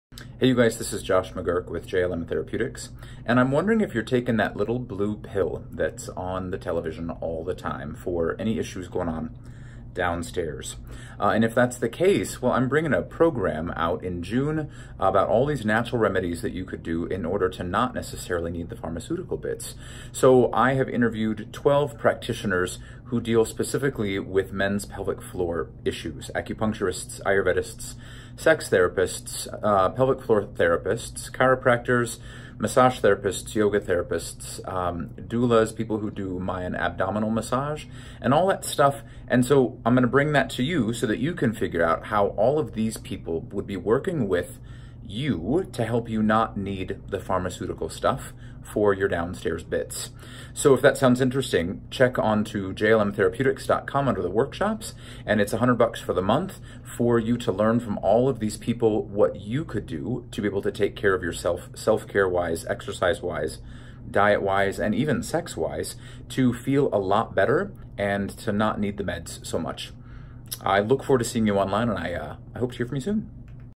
MOPFC Clip 3
Just a little over one minute about MOPFC! I had so much fun filming this clip about men's pelvic floor. You know you're thinking about it, and you're nervous about even clicking Like. to register already!